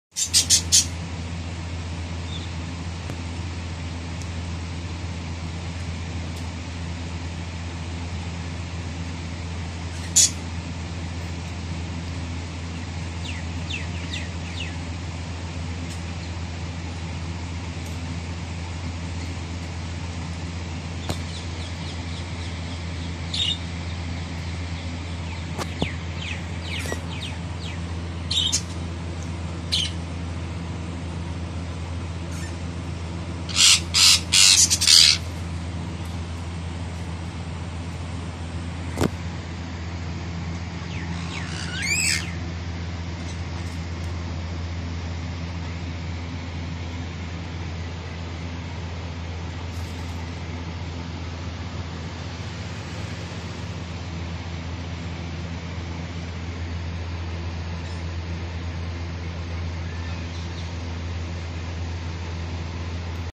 Parrot breakfast, parrots favourite food
Parrot breakfast with rabbit. Parrot food, parrot eating tomato, parrot eating, rabbit diet